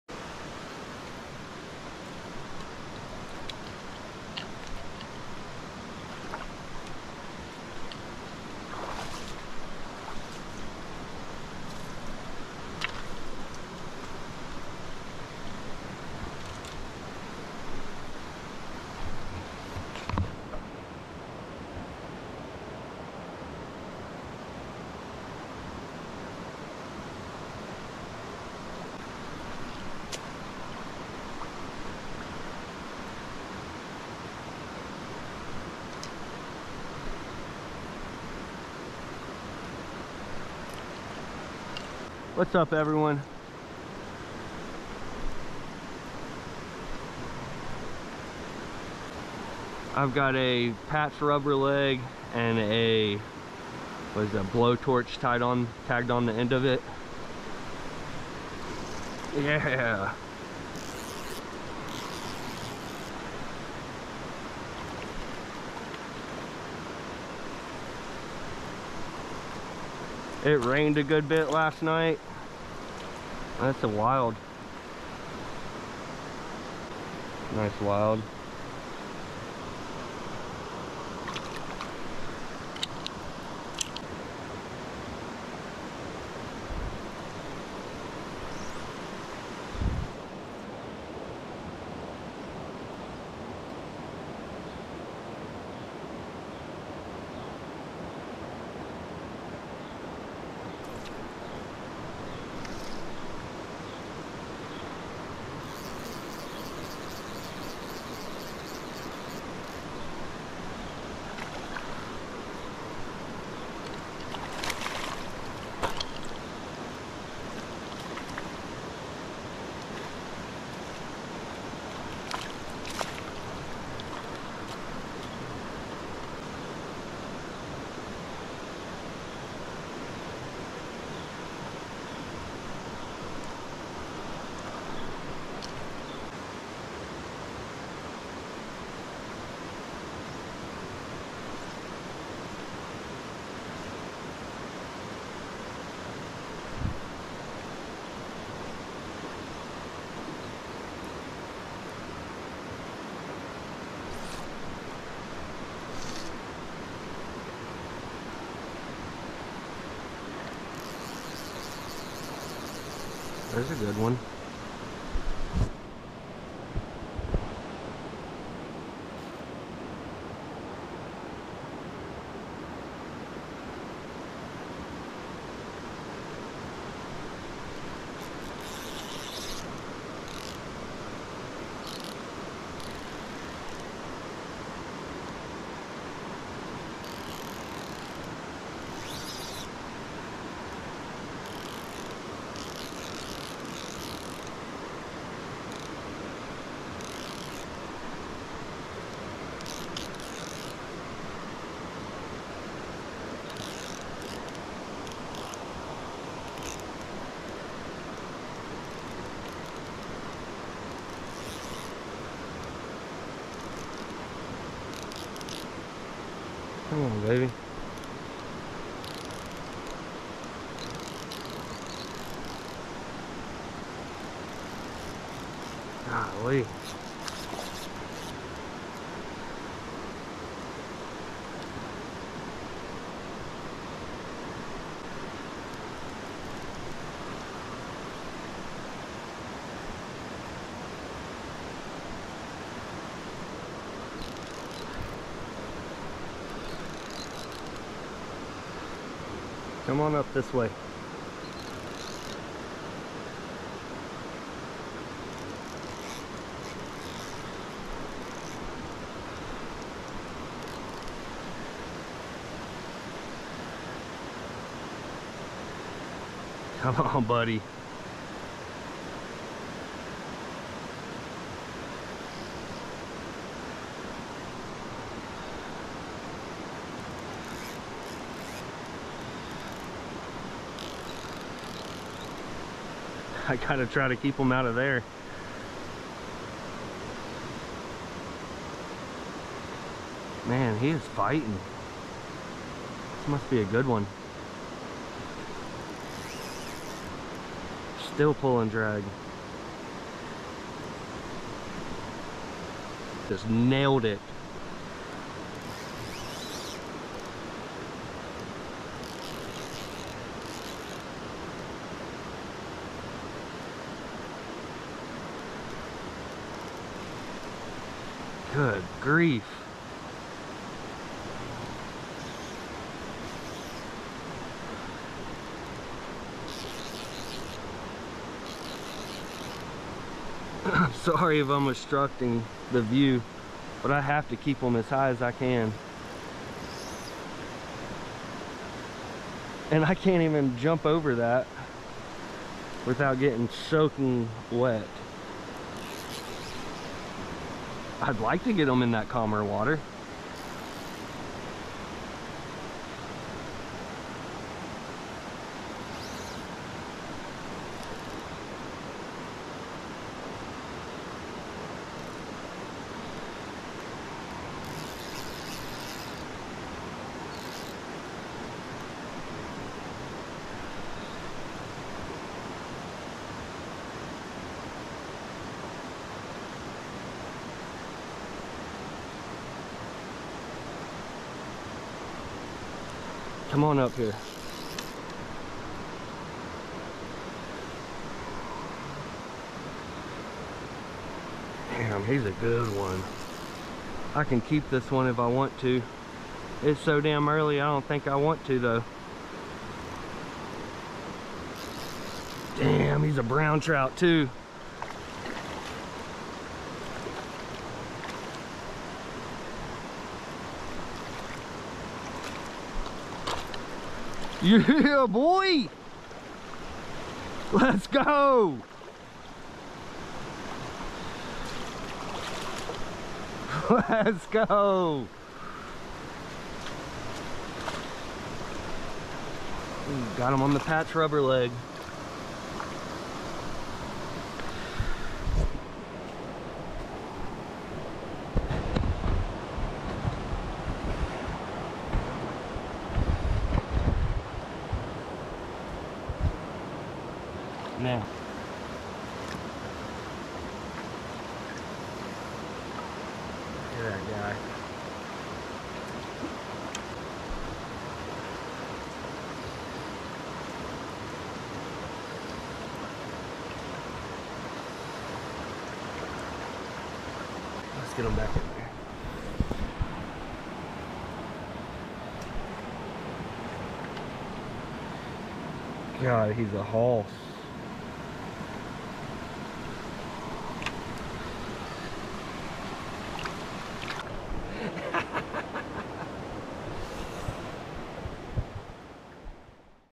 Trophy Trout in WNC Public Waters! Fly Fishing for Trout
Woke up this morning and it was 65° outside! Went to a local stream and had a brown trout pulling drag! I can almost smell Fall in the air.